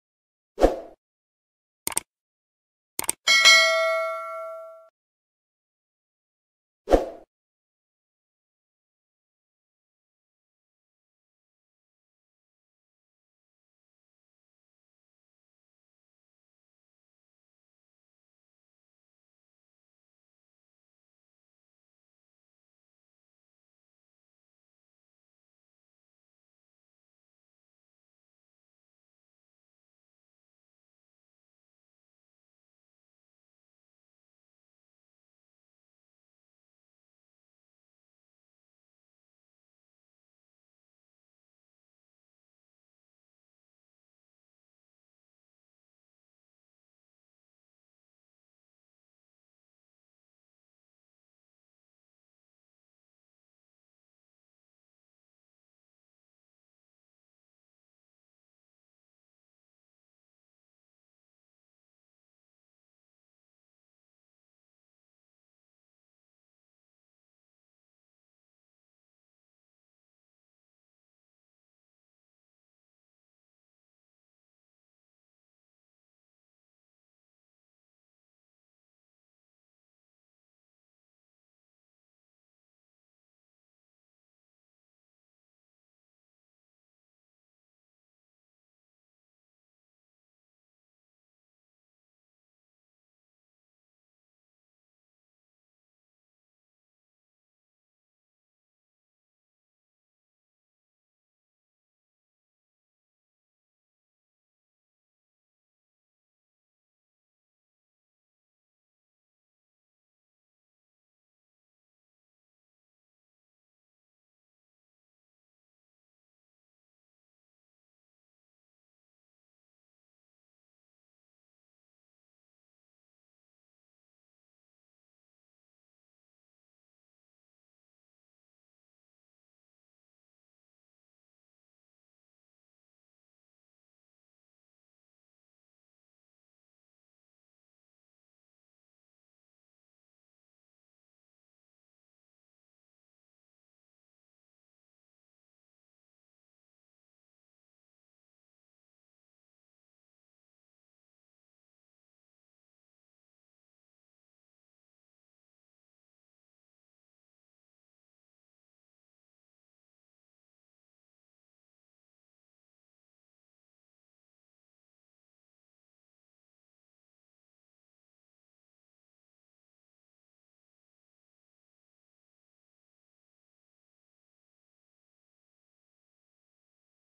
LAAM Anarkali Style | LAAM Anarkali frocks | Bridal frocks
This video is about LAAM Anarkali style frocks designs for girls womens and bridal.